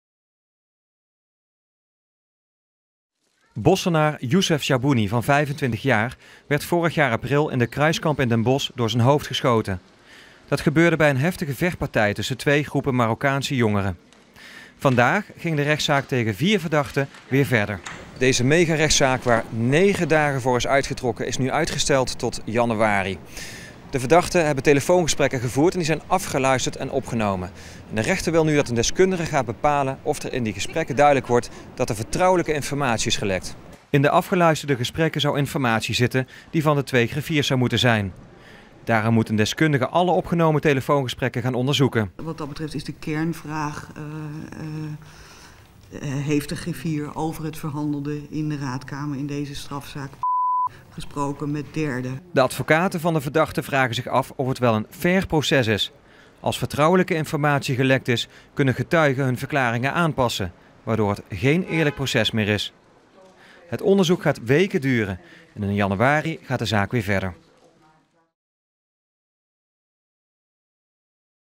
Zaak Chahbouni: verzoek om vrijlating afgewezen
Het verzoek in de zaak Chabouni om vrijlating van de hoofdverdachte, die al 1 jaar en 7 maanden in voorarrest zit, is afgewezen. De zaak wordt in januari voortgezet.